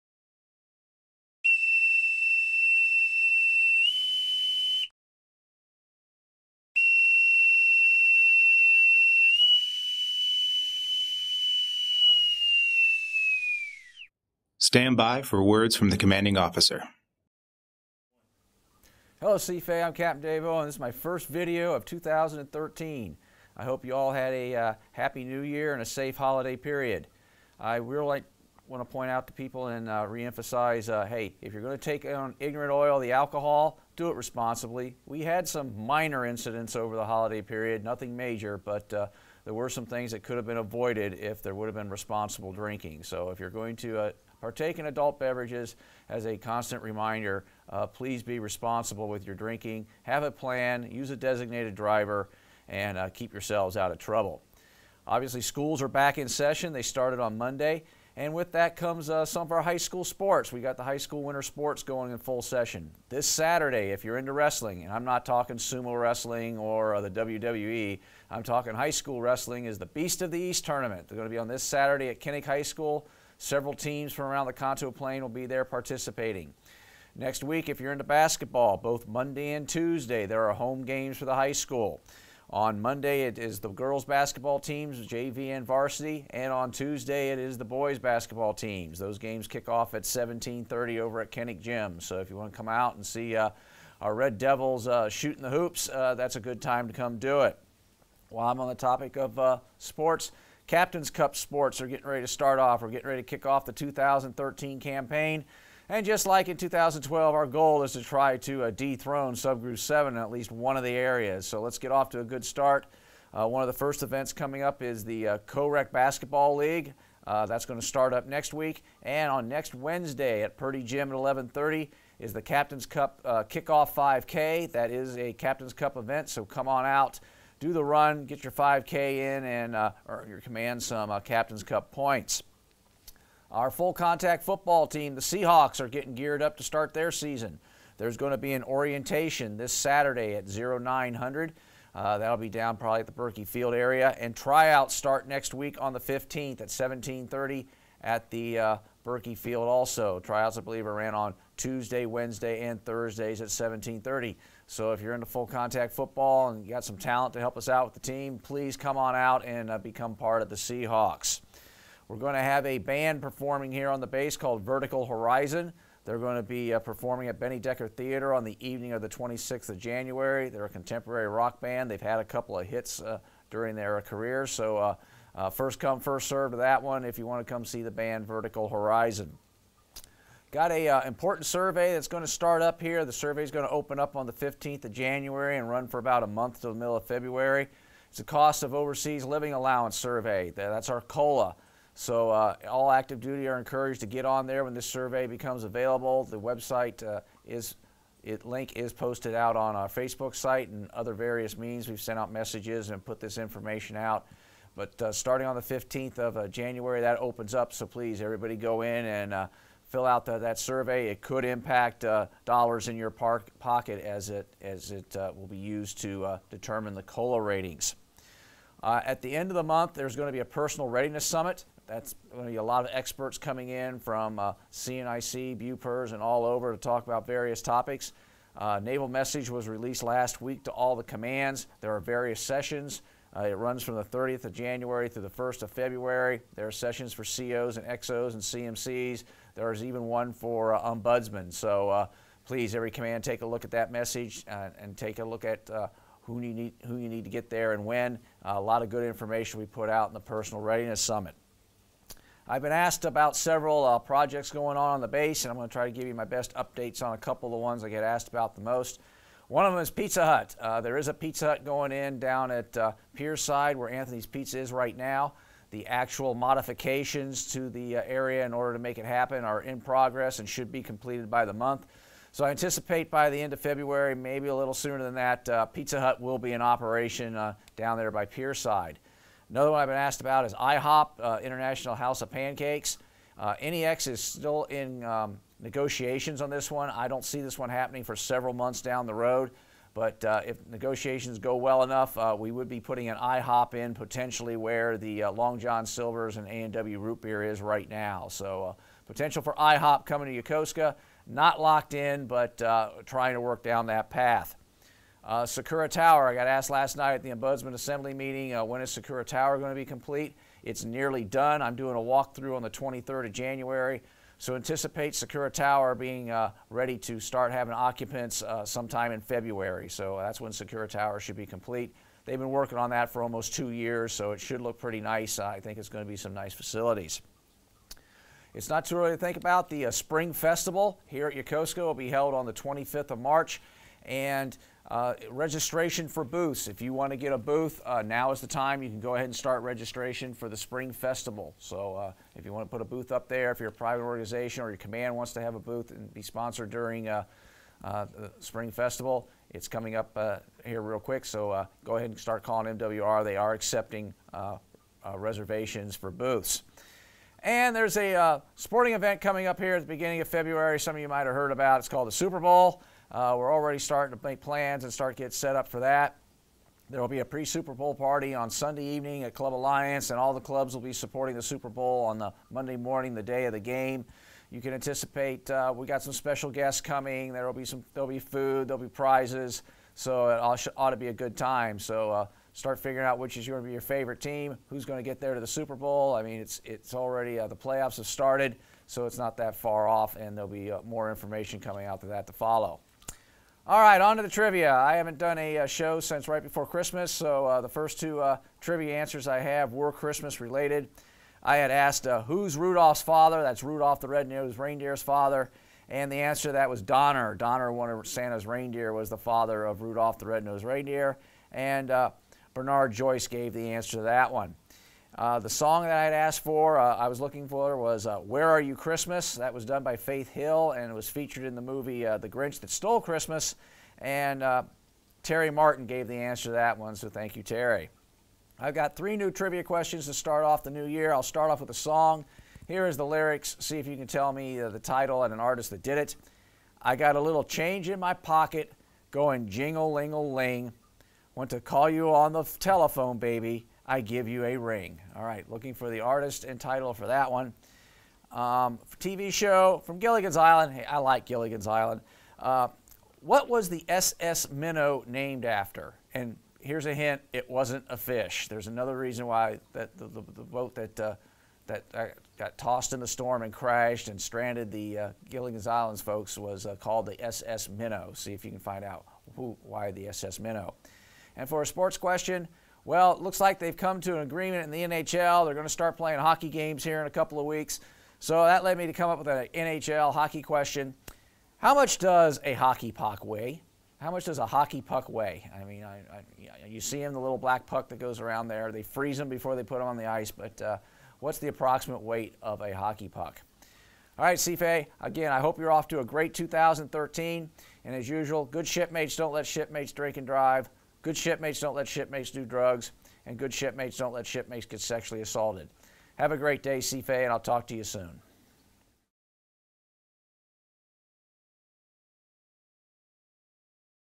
CFAY CO Capt. David Owen's update for January 10, 2013
Topics covered include alcohol incident awareness and prevention, high school sports, captain's cup sports, Seahawk football season, Vertical Horizon concert, COLA survey, personal readiness summit, Pizza Hut construction, possible IHOP constuction, Sakura Tower update, Spring Festival, Super Bowl party, answers to trivia and new trivia questions.